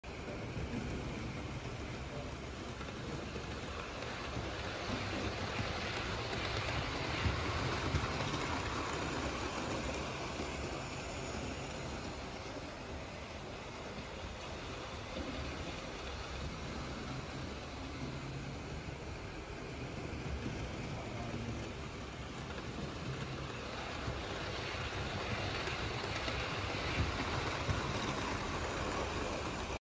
Bulleid Pacific 34088 213 Squadron and Lion out on the society layout
Bulleid pacific and Lion out for a run